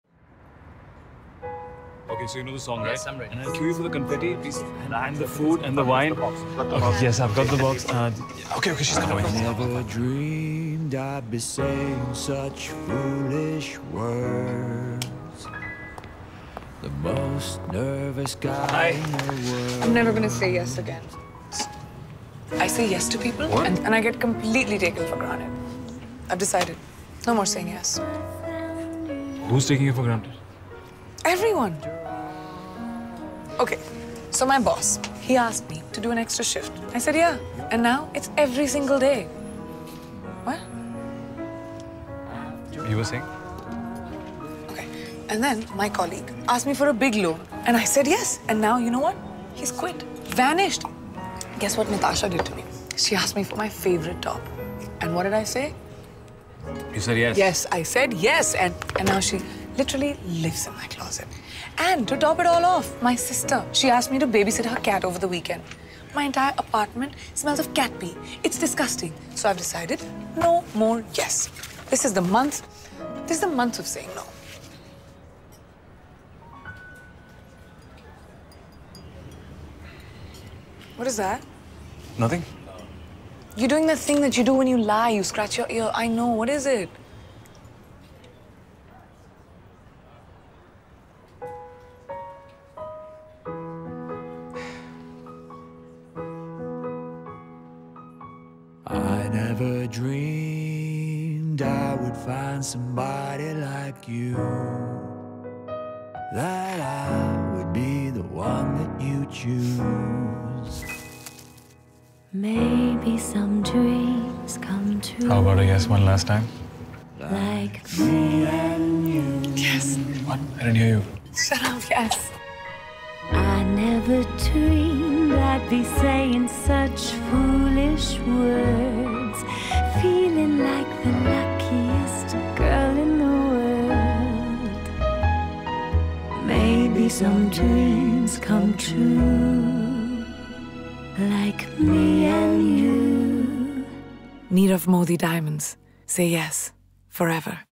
Say yes, forever - Nirav Modi Jewels | Sidharth Malhotra | Priyanka Chopra | Shakun Batra
It's beautiful! Sidharth Malhotra, Priyanka Chopra & Shakun Batra turn on the classic valentine's charm in this NIRAV MODI story.

Starring Sidharth Malhotra & Priyanka Chopra.
Directed by Shakun Batra